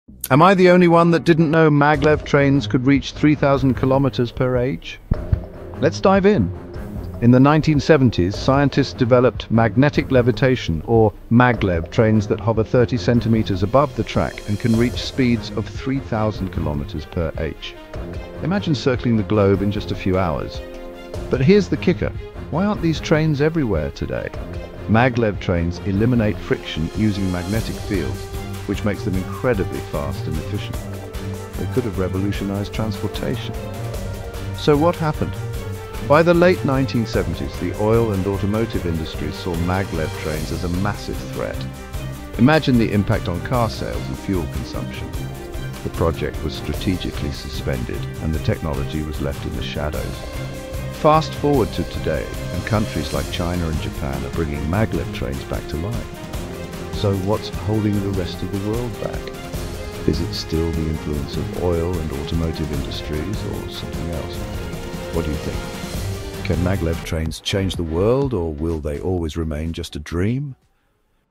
The Hidden Forces Behind Magnetic Levitation: Why Can't We See Them In Use?
Discover the fascinating world of Magnetic Levitation Trains in our latest video, "Magnetic Levitation Trains: The Untold Story." Explore the groundbreaking technology developed in the 1970s that could have revolutionized transportation—imagine trains hovering 30 cm above tracks and reaching speeds of 3000 km/h! But why haven't we seen this world-changing innovation in use today? Join us as we uncover the hidden obstacles posed by the oil and automotive industries, and examine modern advancements that could finally bring this technology to life. Could magnetic levitation trains truly reshape our future, or will they remain a dream?